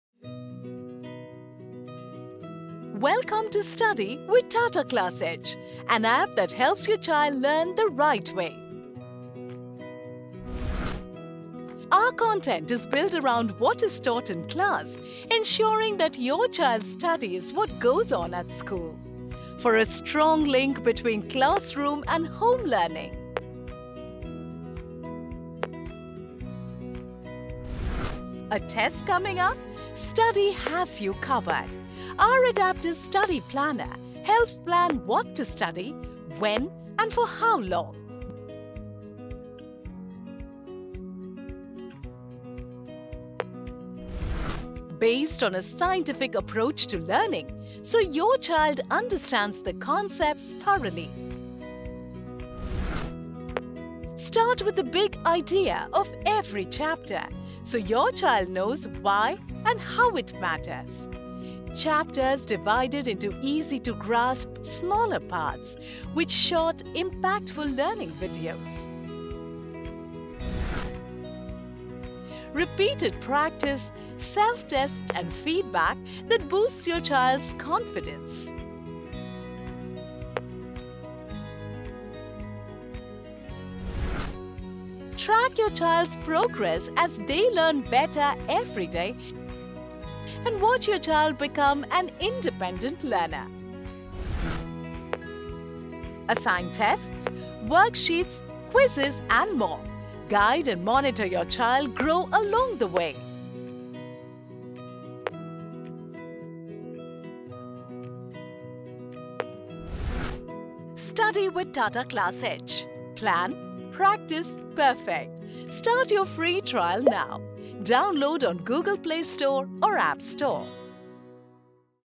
Studi with TCE | READY? STUDi GROW!
Introduce your child to Studi and discover the Science of learning. Watch how your child develops efficient study habits.
Start your journey today! Ready? Studi, Grow!